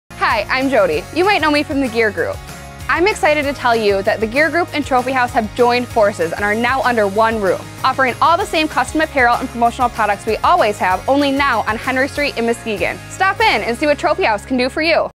15 Second Commercial Spot
Trophy House & Jones Sports have joined up with Gear Group. This is our 15 second commercial spot to announce our team up.